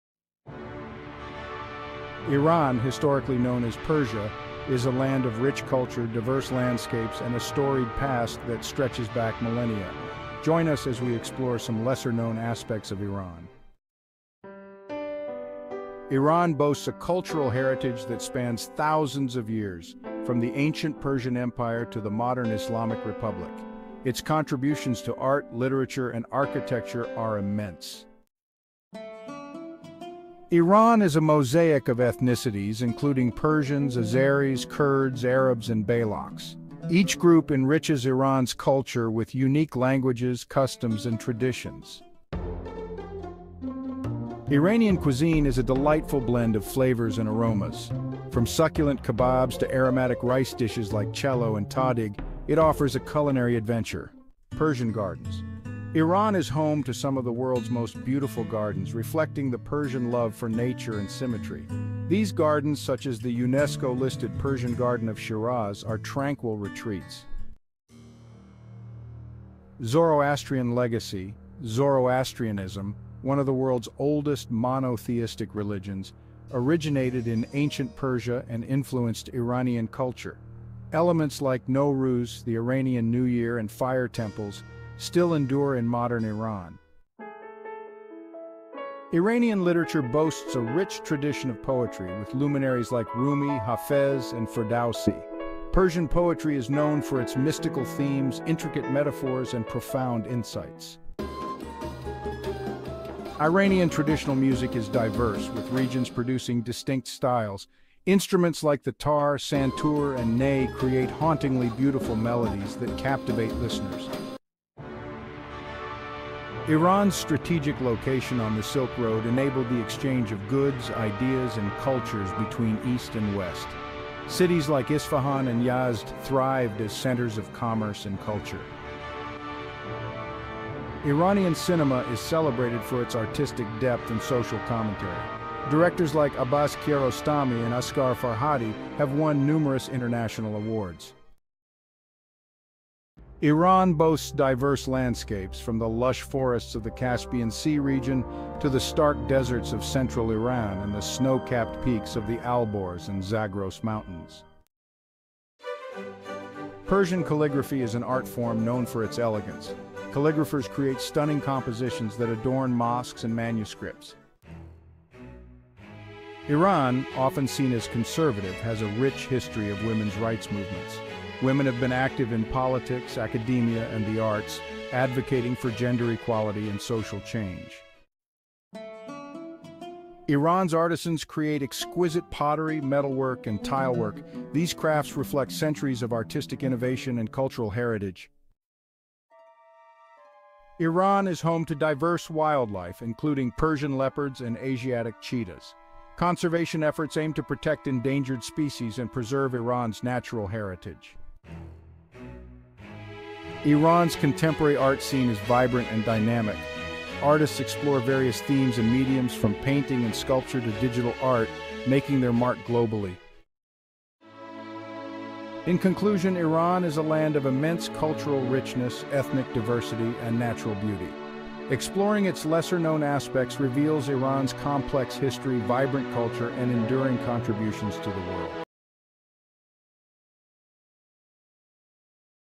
Explore Iran's Incredible Secrets 🇮🇷
Iran, also known as Persia, is a land of rich culture, diverse landscapes, and a storied past that spans thousands of years. Its cultural heritage includes the ancient Persian Empire to the modern Islamic Republic, with contributions to art, literature, music, and architecture being immense. Iran's ethnic diversity is diverse, with Persians, Azeris, Kurds, Arabs, Balochs, and others contributing to its cultural tapestry through their language, customs, and traditions.

Iranian cuisine offers a blend of flavors and aromas, influenced by Persian, Arab, Turkish, and Central Asian culinary traditions. The Persian Gardens, such as the UNESCO-listed Persian Garden of Shiraz, reflect the Persian love for nature and symmetry. Zoroastrianism, one of the world's oldest monotheistic religions, originated in ancient Persia and profoundly influenced Iranian culture. Elements of Zoroastrianism, such as the celebration of Nowruz (Iranian New Year) and fire temples, still endure in modern Iran.

Poetry and literature in Iran are rich, with luminaries like Rumi, Hafez, and Ferdowsi leaving an indelible mark on world literature. Traditional music in Iran is diverse, with different regions producing distinct musical styles and instruments. Iran's strategic location at the crossroads of ancient trade routes, including the Silk Road, facilitated the exchange of goods, ideas, and cultures between East and West.

Iranian cinema has gained international acclaim for its artistic depth and social commentary. Filmmakers like Abbas Kiarostami, Asghar Farhadi, and Jafar Panahi have won numerous awards at prestigious film festivals worldwide. Natural wonders in Iran include the Caspian Sea region, central Iran, and the Alborz and Zagros Mountains.

Persian calligraphy is an elegant and beautiful art form, with calligraphers crafting visually stunning compositions for mosques and manuscripts. Women's rights movements in Iran have been active, pushing for greater gender equality and social change. Traditional crafts in Iran include pottery, metalwork, miniature painting, and tilework, reflecting centuries of artistic innovation and cultural heritage.

Lastly, Iran is home to a diverse array of wildlife, including Persian leopards, Asiatic cheetahs, and Persian fallow deer, with conservation efforts underway to protect endangered species and preserve Iran's natural heritage.

Iran's contemporary art scene is dynamic and diverse, featuring artists exploring various themes and mediums. From painting and sculpture to digital art and performance, Iranian artists are making a significant impact on the global art world. Iran's rich cultural heritage, ethnic diversity, and natural beauty make it an attractive destination for exploring its complex history and enduring contributions.